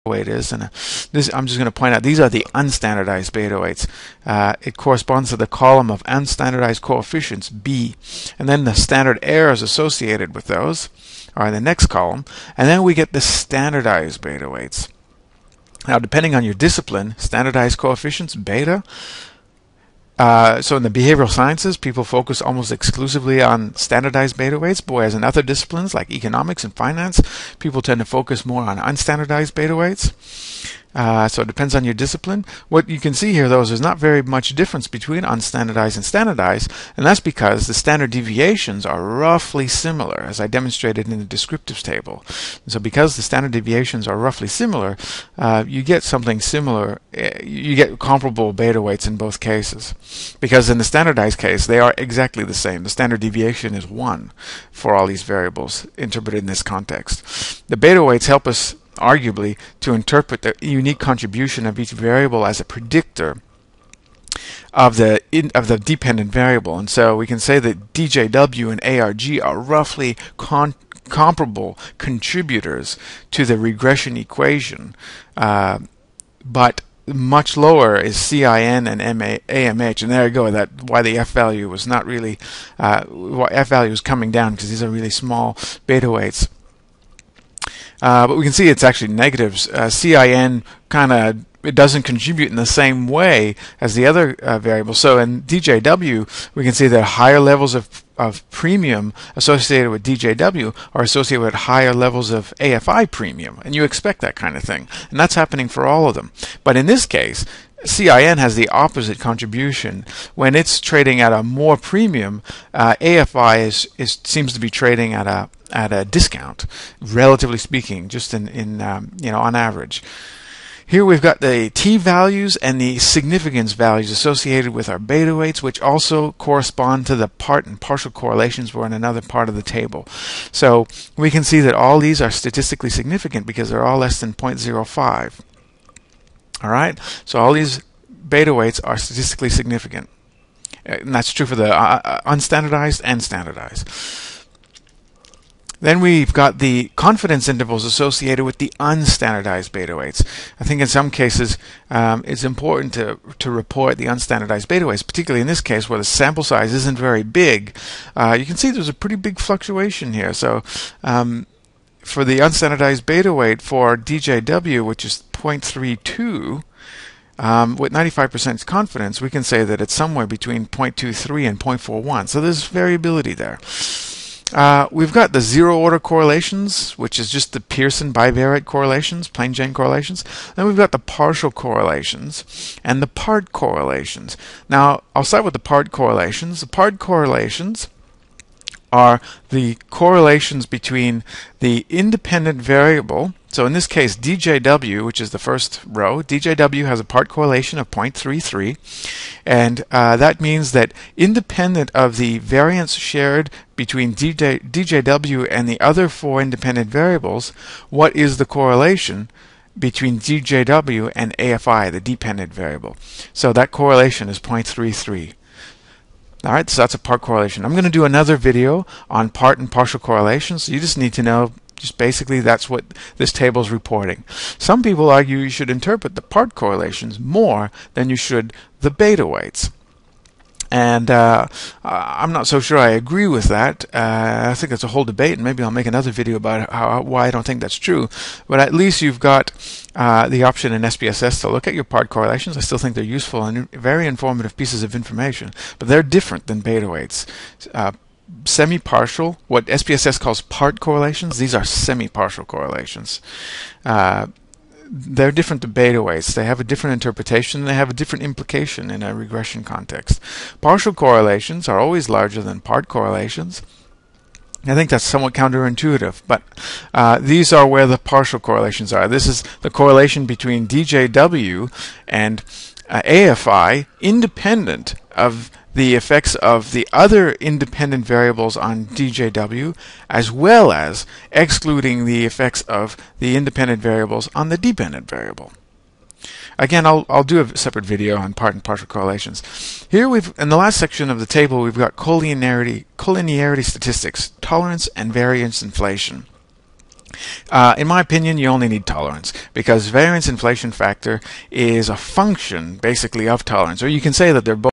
Multiple Regression - SPSS (part 4)
I demonstrate how to perform a multiple regression in SPSS. This is the in-depth video series. I cover all of the main elements of a multiple regression analysis, including multiple R, R squared, model development (via stepwise method), intercept, unstandardized beta weights, standardized beta weights, semi-partial correlation, standard errors, as well as basic heteroscedasticity tests.